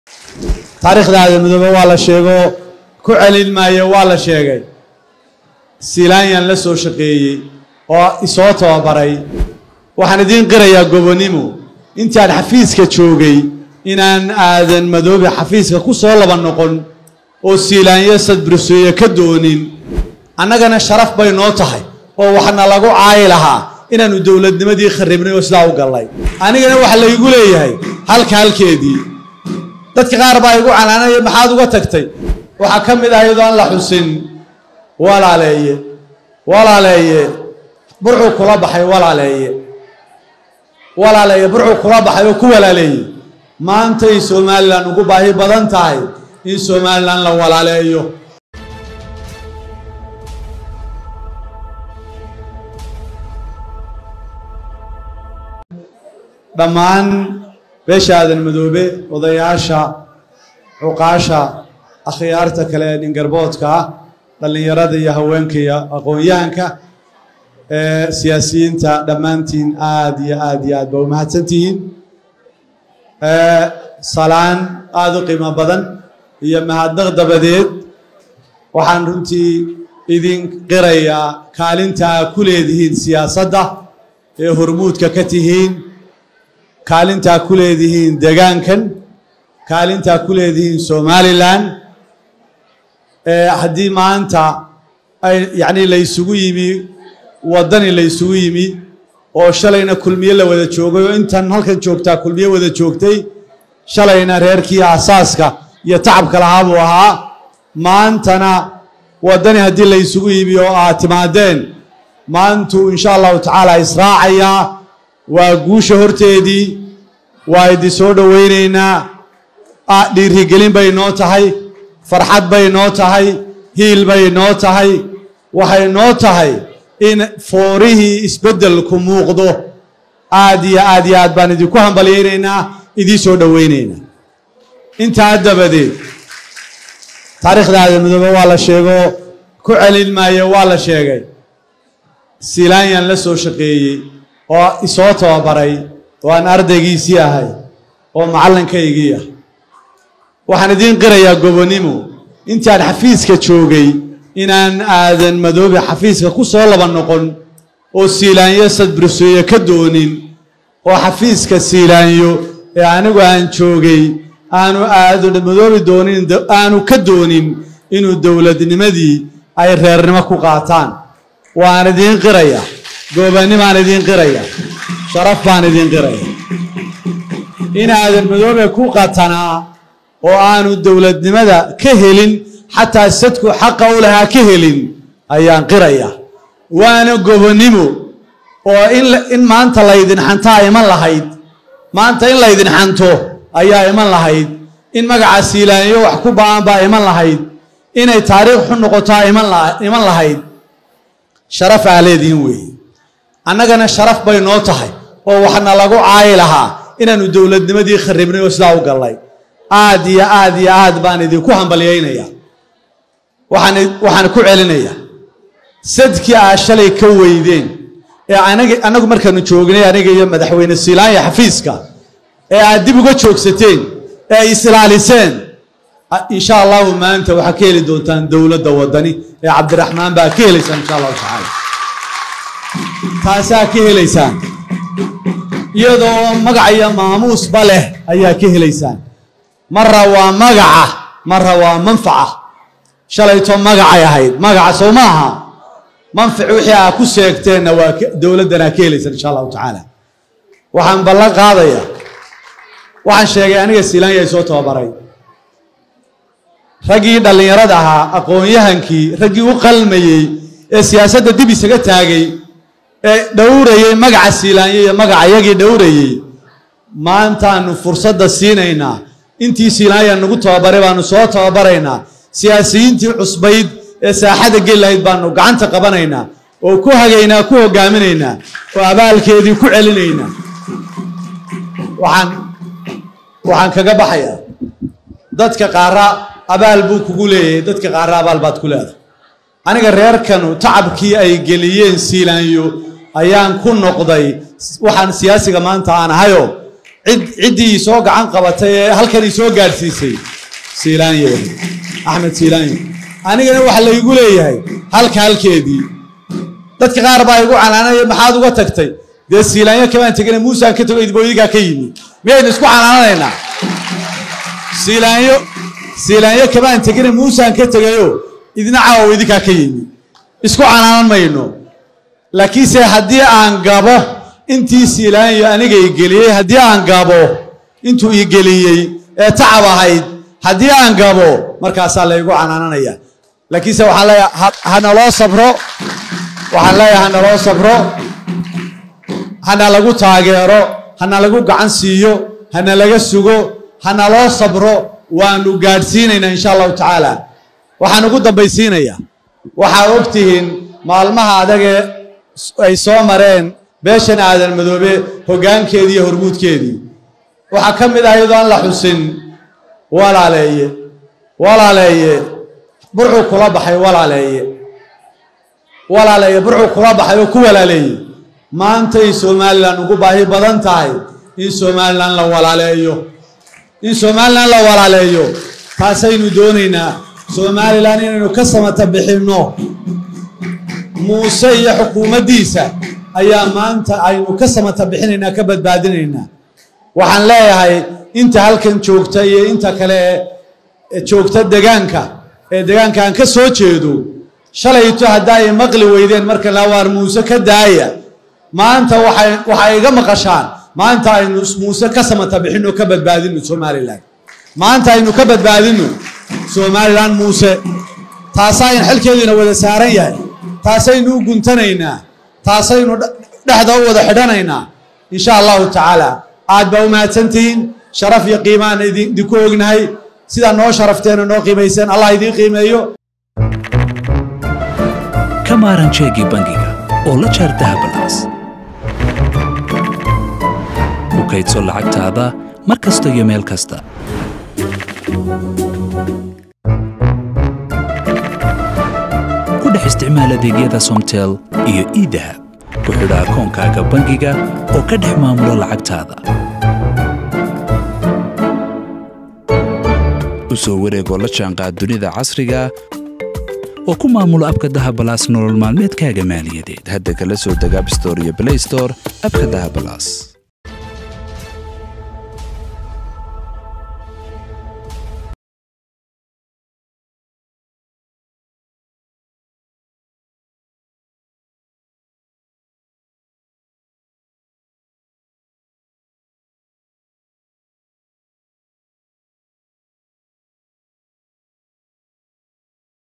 Guddoomiye Xirsi Oo 8 Sanno Kaddib Qiray Sababtii uu Siilaanyo Ugu Wacad-furay
Guddoomiye Xirsi Oo 8 Sanno Kaddib Qirtay Sababtii uu Siilaanyo Ugu Wacad-furay